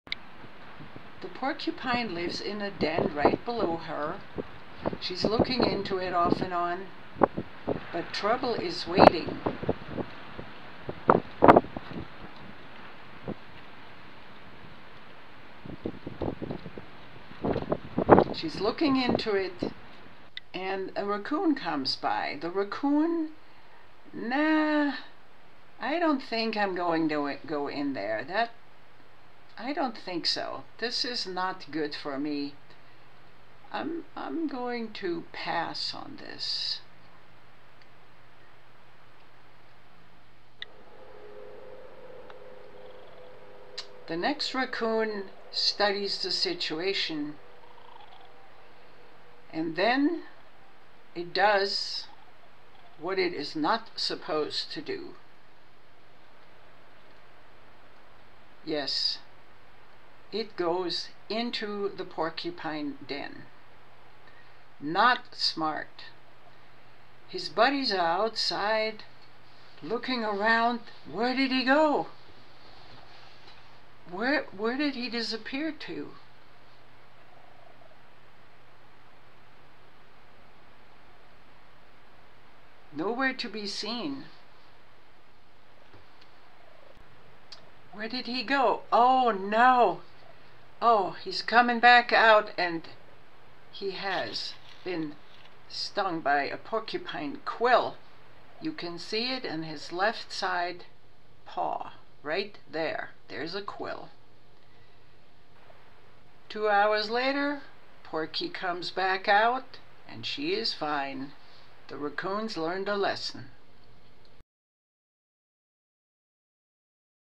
When a raccoon dives into an occupied porcupine den
Turn up your sound so you can hear the story told. Raccoons check out the porky den. The smart coon walks off, there is another one though that's too curious. Watch to the end and see what happens.